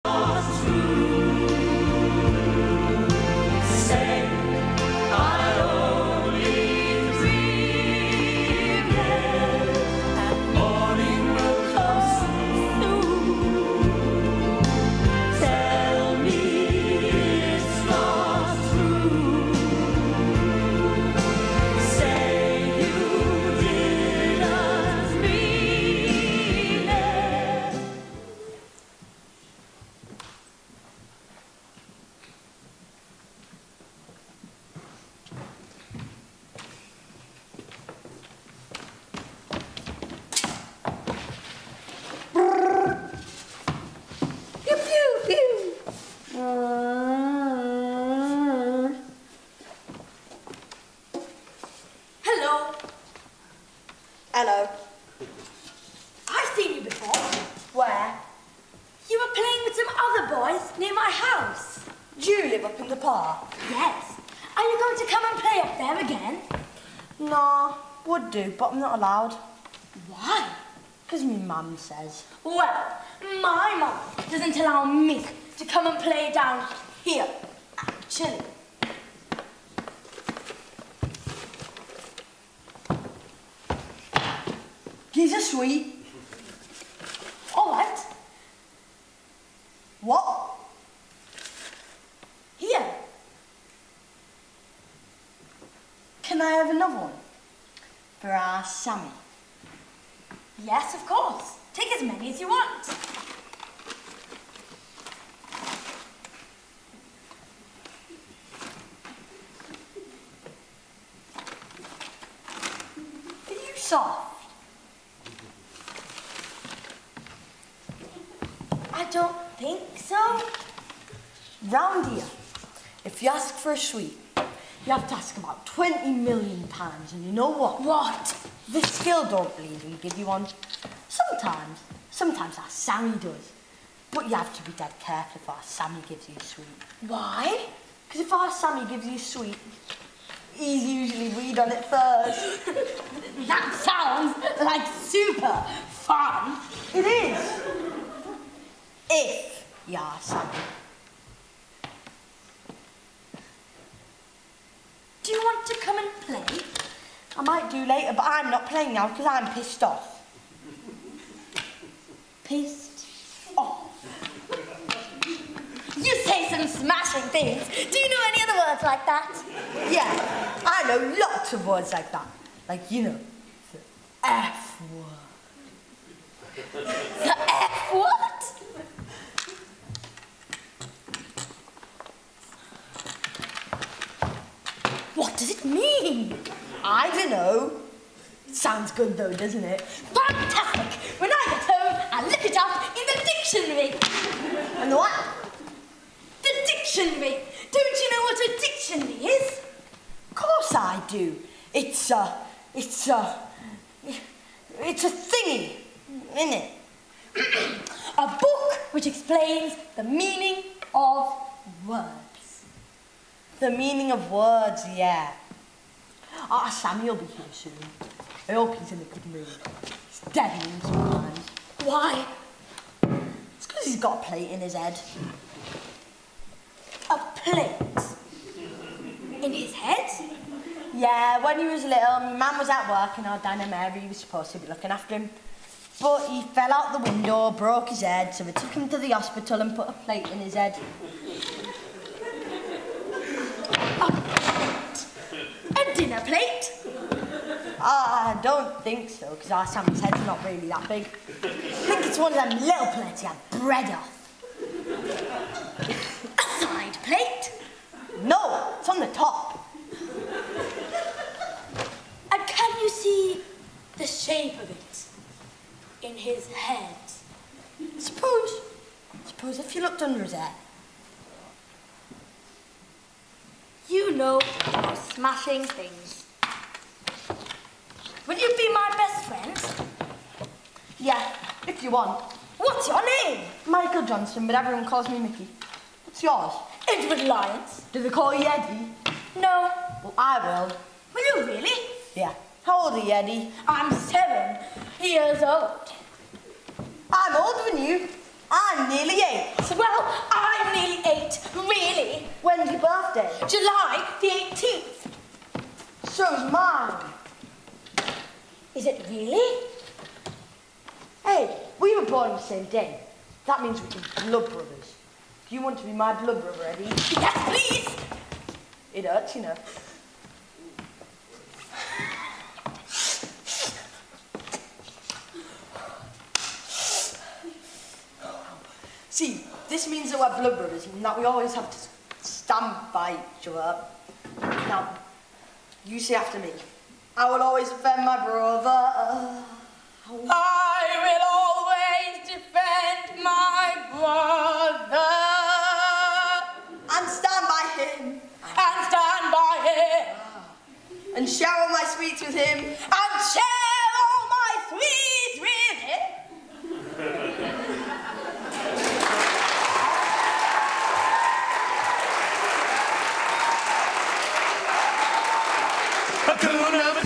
Blood Brothers
Shourjo Dasgupta and Vihaan Mitra's WINNING entry in the Yarm School Junior Duologues competition